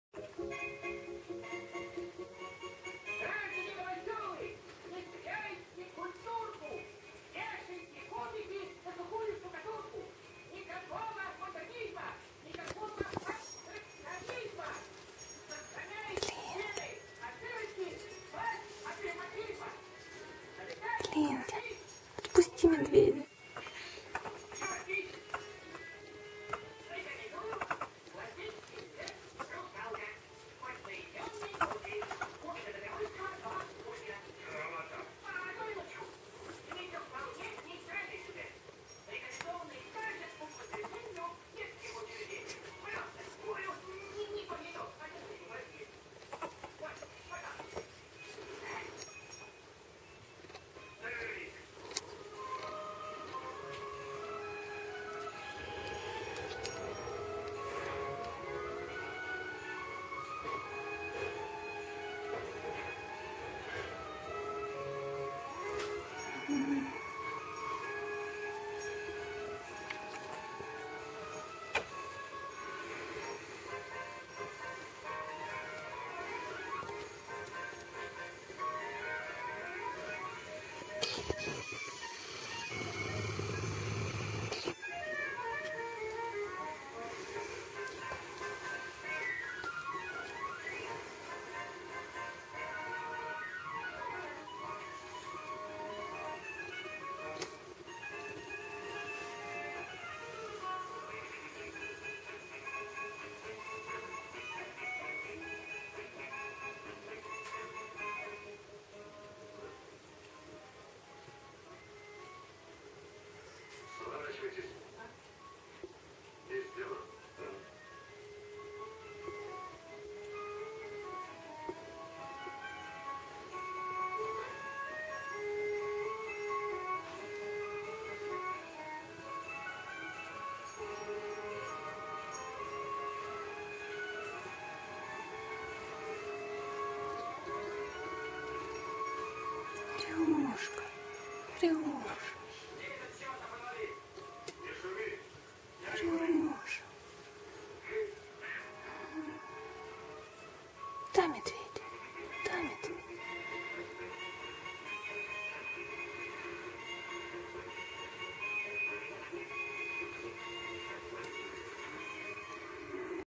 Экстаз у хорька (Слинки)
Вот за таким делом застукала своего любимца.. думала он уснул с игрушкой в зубах., взяла камеру, да не так всё просто там было.. гормоны уже бушуют во всю!